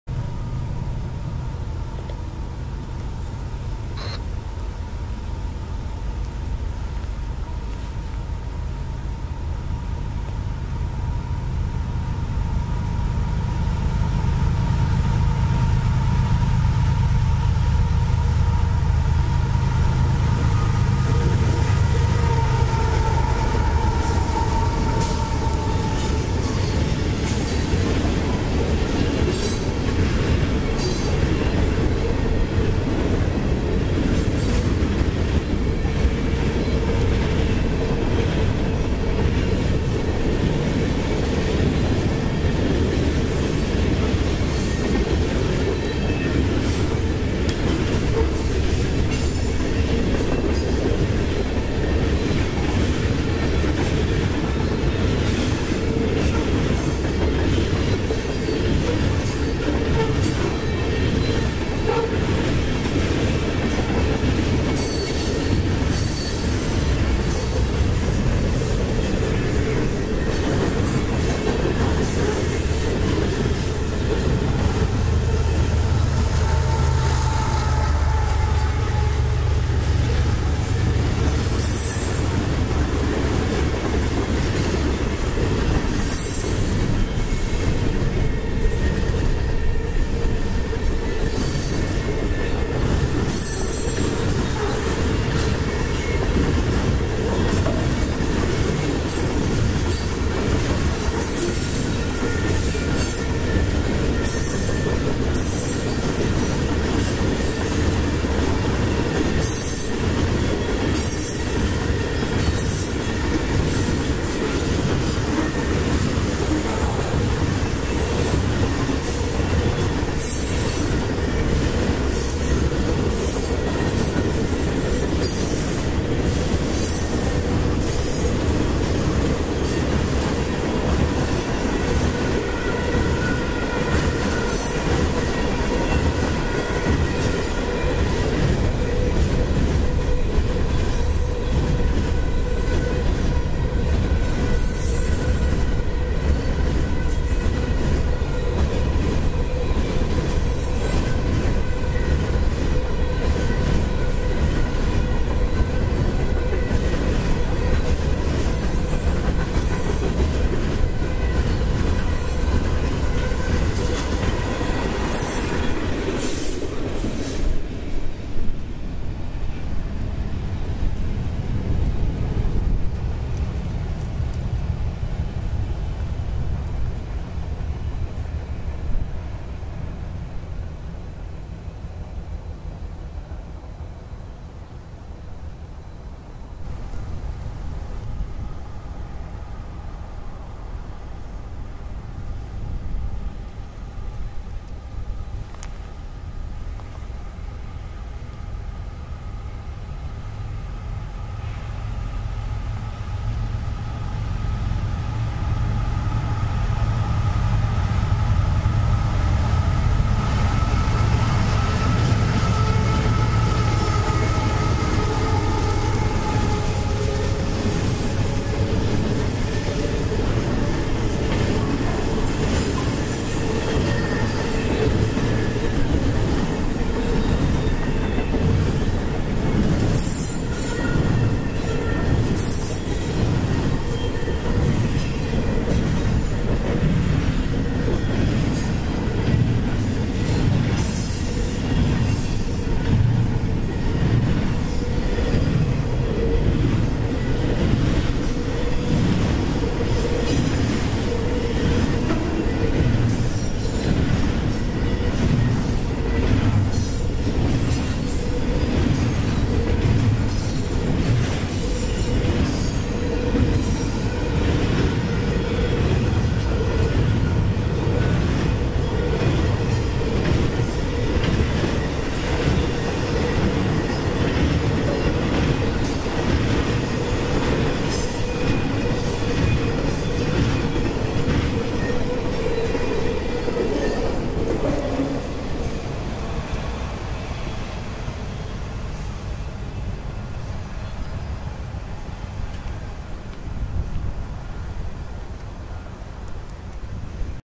CP AC44CW 9617 trailing on Q-022 Friday March 5th 2021.
We have a big Q-436 followed by Q-022 With a visitor from the Great White North today. Both trains passed within 10 minutes of each other.